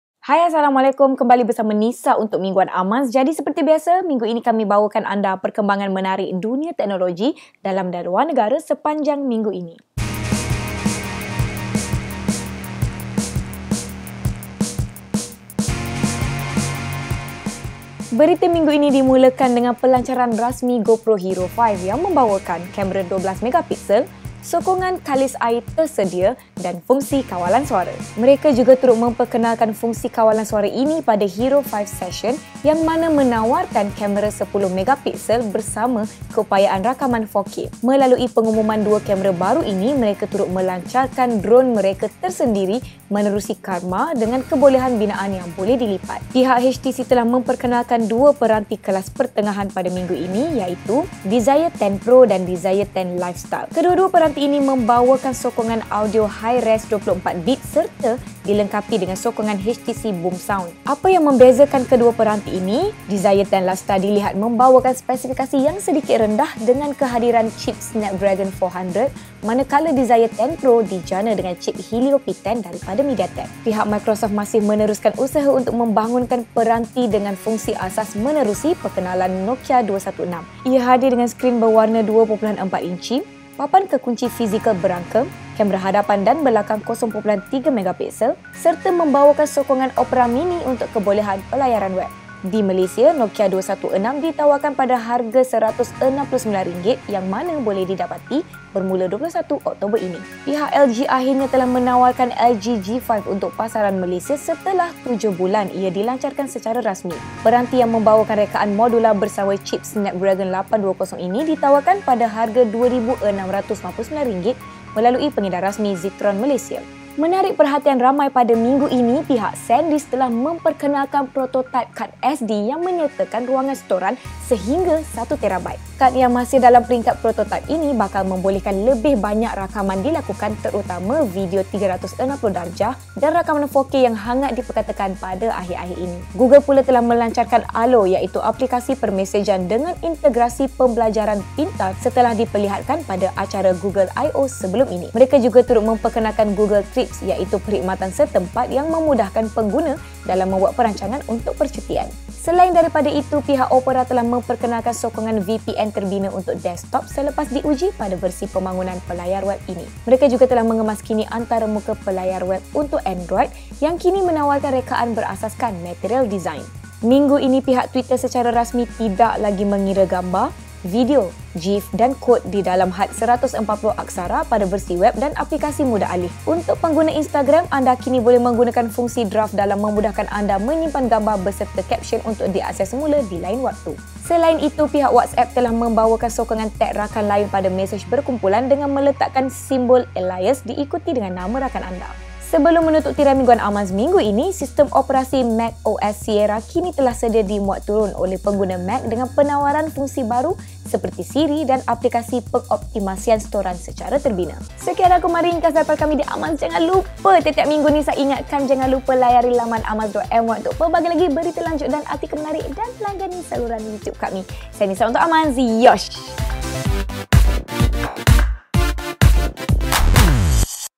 Mingguan Amanz - GoPro Karma, Kad SD 1TB, Google Allo
Minggu ini menyaksikan pihak GoPro mencuri perhatian dengan memperkenalkan kamera aksi terbaru mereka, iaitu GoPro Hero5 yang kini turut hadir dengan sokongan rakaman video pada resolusi 4K, selain menyertakan sokongan kalis air terbina dan hadir dengan kebolehan pengawalan berasaskan kepada arahan suara.

Bersama-sama Hero5, pihak GoPro turut memperkenalkan dron pertama mereka dinamakan sebagai GoPro Karma. HTC turut memperkenalkan dua peranti baru dibawah HTC Desire, iaitu HTC Desire 10. Seperti biasa, ia menawarkan spesifikasi pertengahan, tetapi dengan rekaan elegan seumpama HTC 10 sebelum ini.

Turut mencuri perhatian adalah pengenalan aplikasi permesejan pintar daripada Google, iaitu Google Allo. Aplikasi ini menawarkan kebolehan permesejan bersama-sama rakan-rakan yang hadir dengan pembelajaran pintar dan turut boleh memanggil bantuan Google Assistant daripada sebarang tetingkap.

Opera pula pada minggu ini telah memperkenalkan integrasi OperaVPN pada pelayar web desktop versi stabil mereka, disamping turut menyertakan antaramuka baru untuk Opera Android.

Dan akhir sekali, turut mencuri perhatian adalah pengenalan kad mikro-SD 1TB daripada Sandisk, yang mana masih pada peringkat prototaip awal buat masa ini.